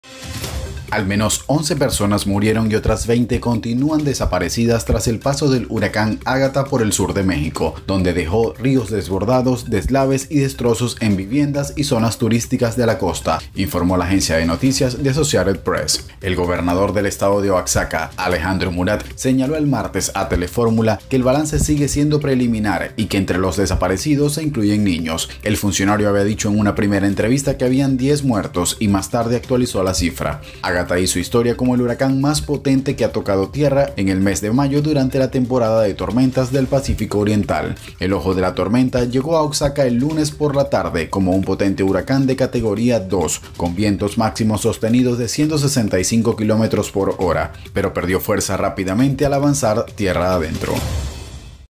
TRAGEDIA: DECENAS DE MUERTOS Poderoso huracán Ágatha deja fatídico saldo y múltiples desaparecidos
TRAGEDIA EN MIÉRCOLES: DECENAS DE MUERTOS. Poderoso huracán Ágatha deja fatídico saldo y múltiples desaparecidos; advierten a dónde se dirige